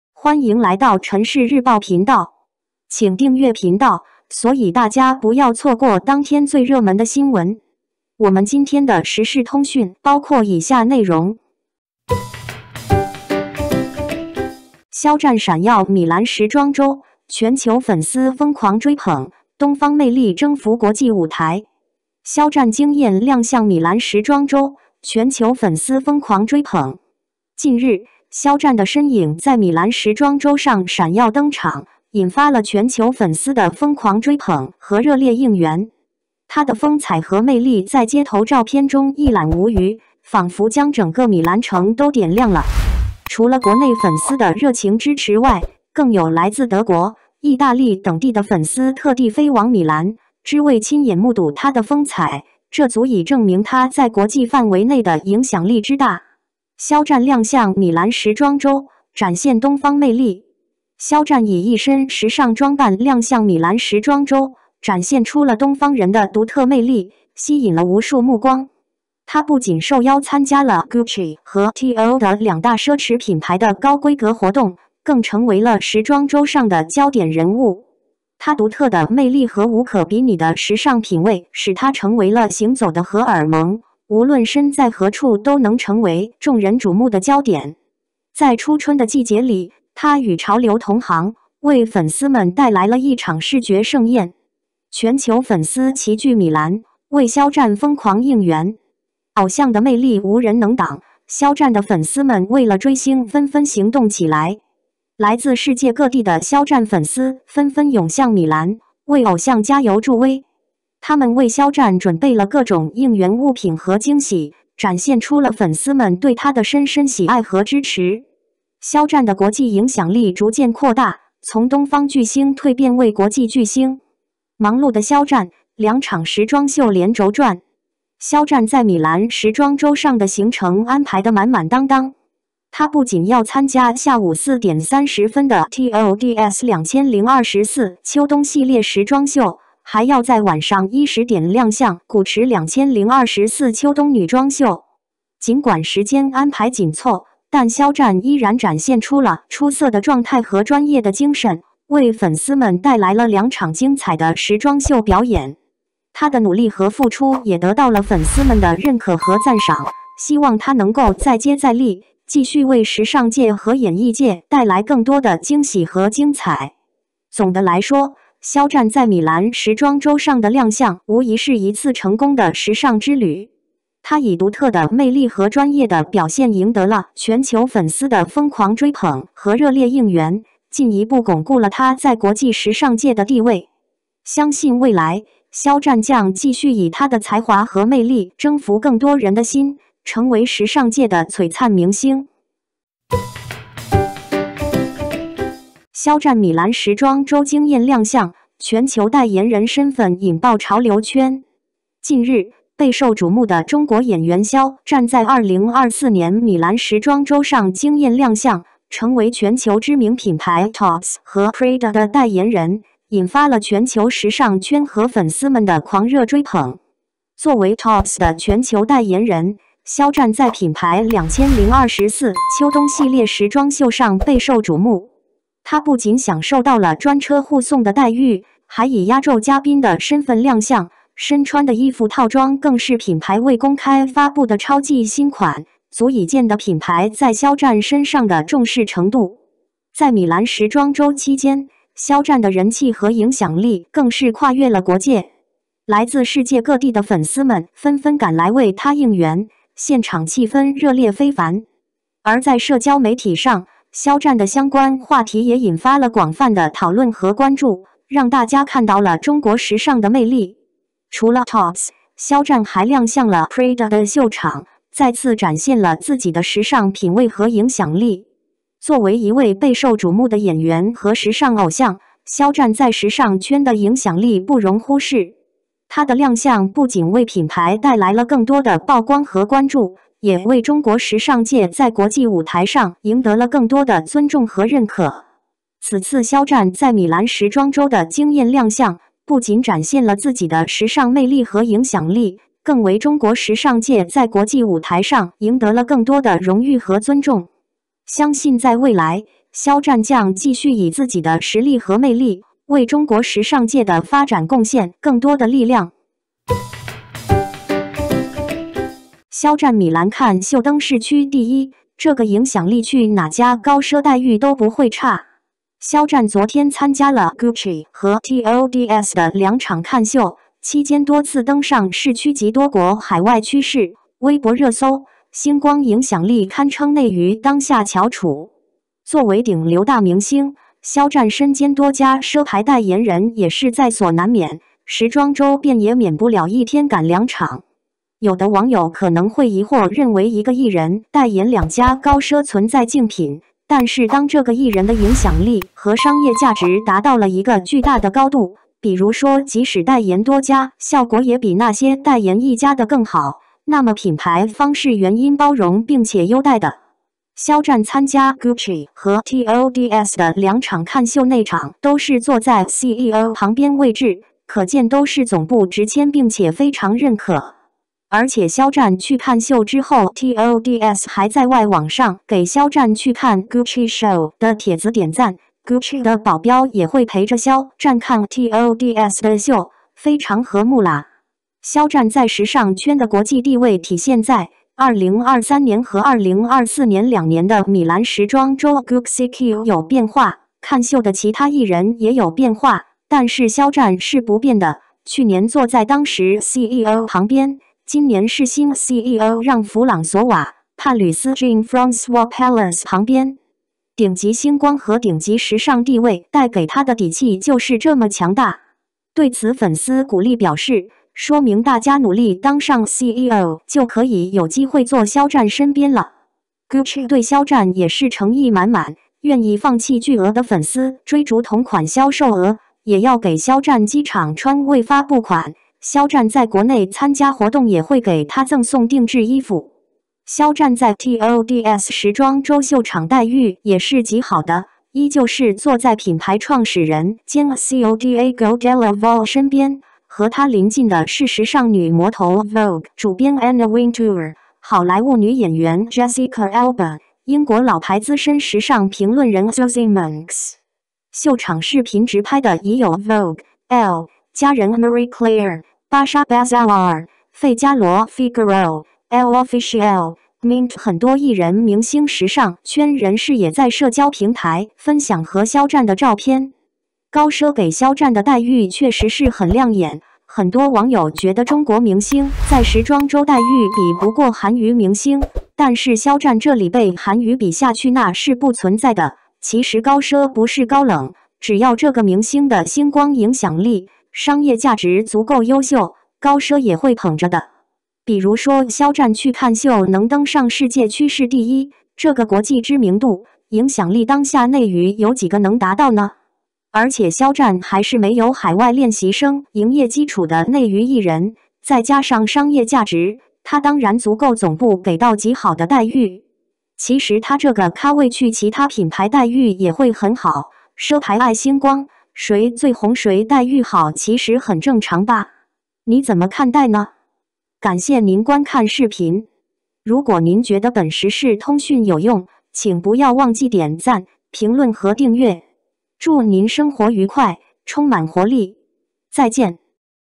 【陈氏日报2月24日】肖战 闪耀米兰时装周：全球粉丝疯狂追捧，东方魅力征服国际舞台，这个影响力去哪家高奢待遇都不会差#xiaozhan
肖战闪耀米兰时装周：全球粉丝疯狂追捧，东方魅力征服国际舞台！
独家揭秘！肖战米兰时装周惊艳亮相，全球代言人身份引爆潮流圈
肖战米兰看秀登世趋第一，这个影响力去哪家高奢待遇都不会差